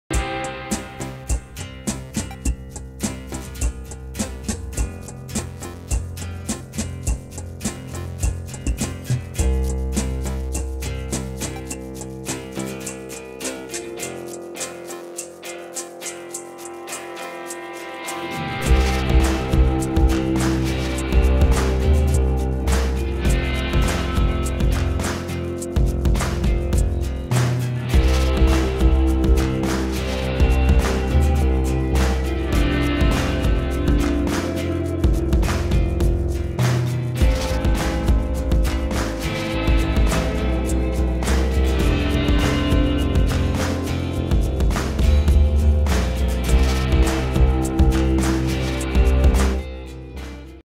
Carta delle Insalate - Ricetta Olive con emmenthal e mele al curry
Voglia di insalatona? Prova questo mix di lattughini, olive e carote, già pronto per essere versato in ciotola insieme ai tuoi ingredienti preferiti. Gli Chef in Camicia hanno aggiunto emmenthal e mele al curry... risultato squisito!